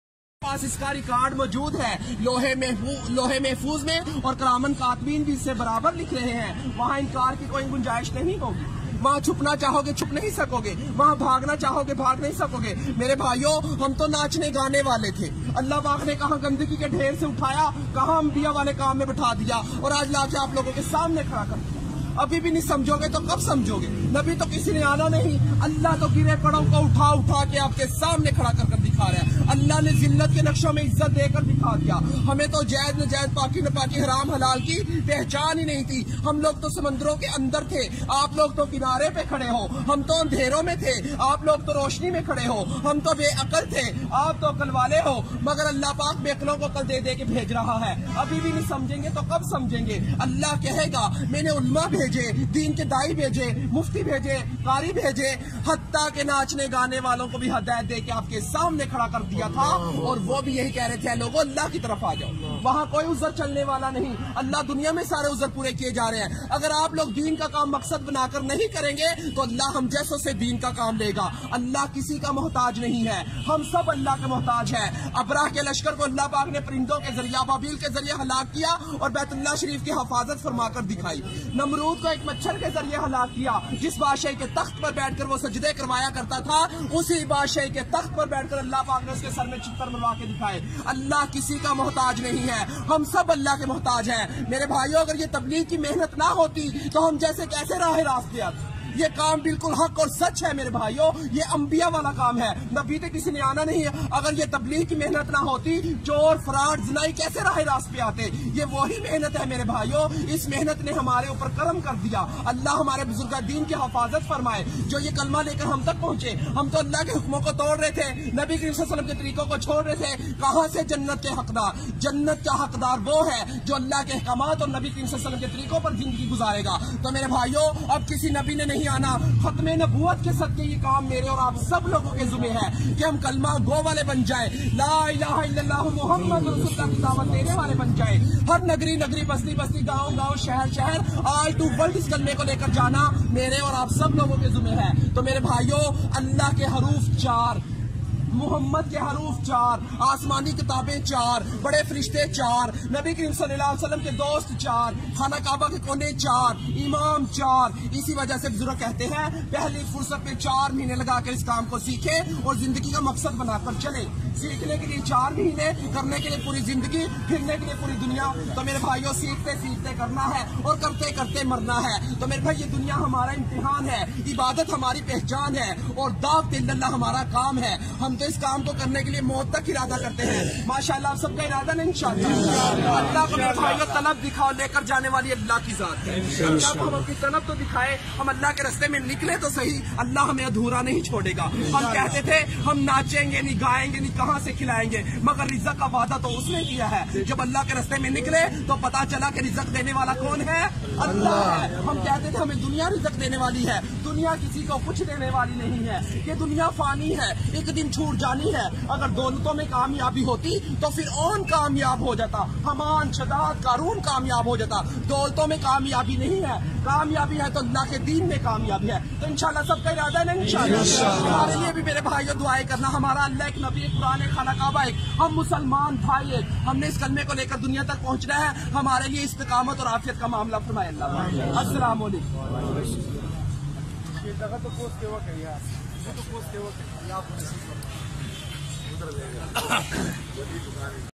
Khwaja Sara (Hijra) invites people toward Islam..Dawat O Tableegh
In this Video Khwaja Sira or Hijra or Khusra invites people toward Islam and tallking about ALLAH and Prophet